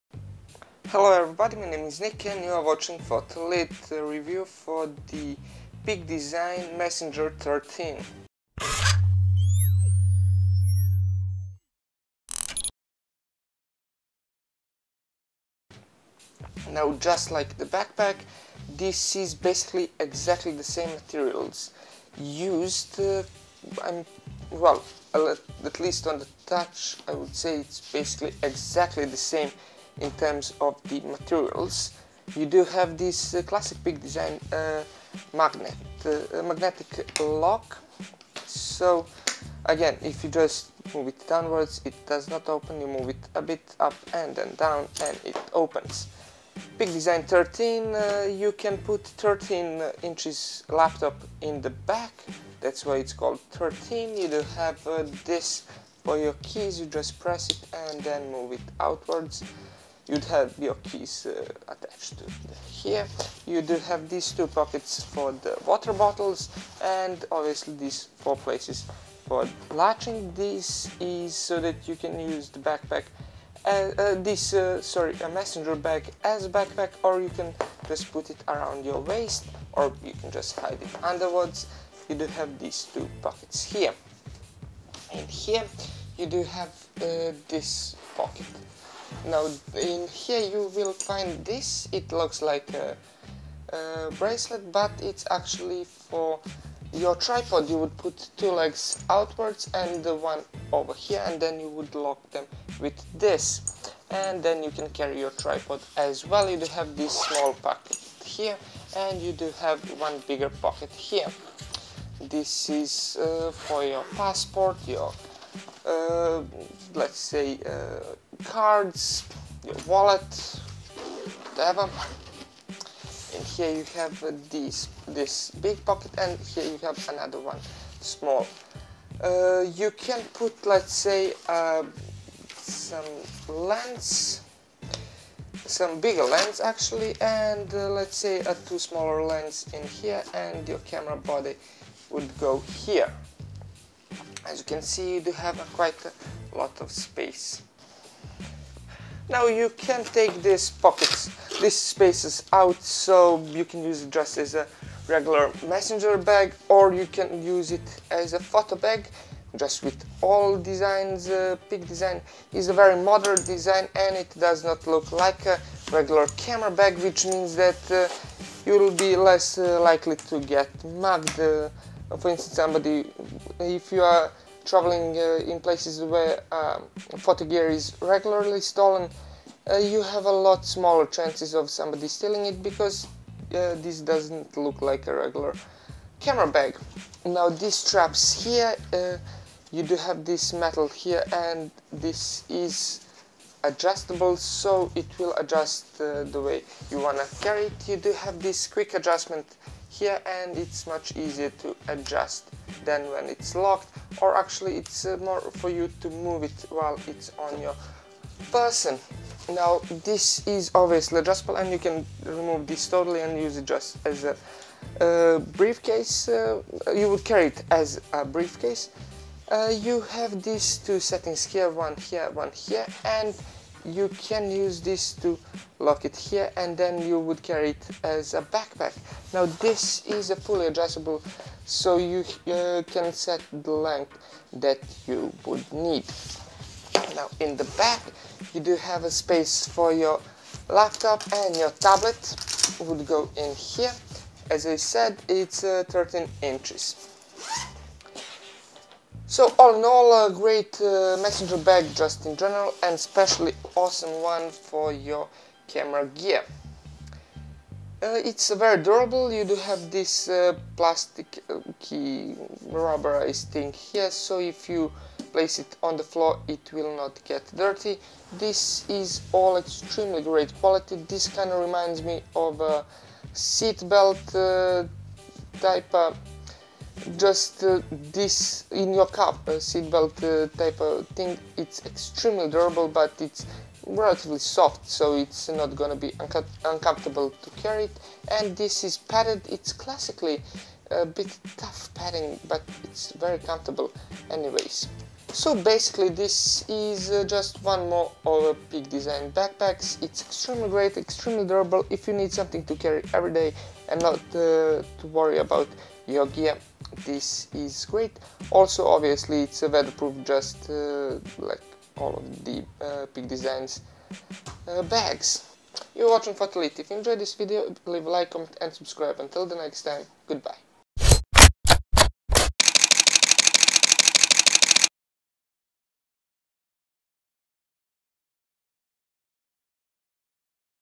Peak Design Everyday Messenger 13 (Charcoal)
Designed for the photographer, traveler, urban commuter, and everyone in between, the charcoal Everyday Messenger 13" from Peak Design showcases smart features to meet your day-to-day needs. The Messenger holds a full-frame DSLR and up to two lenses, thanks to the two origami-inspired adjustable touch-fastening padded dividers. Simply remove the dividers if you prefer to carry books or overnight essentials. Your up to 13" laptop can be stored separately in a rear, padded compartment with an interior sleeve for your tablet or important documents.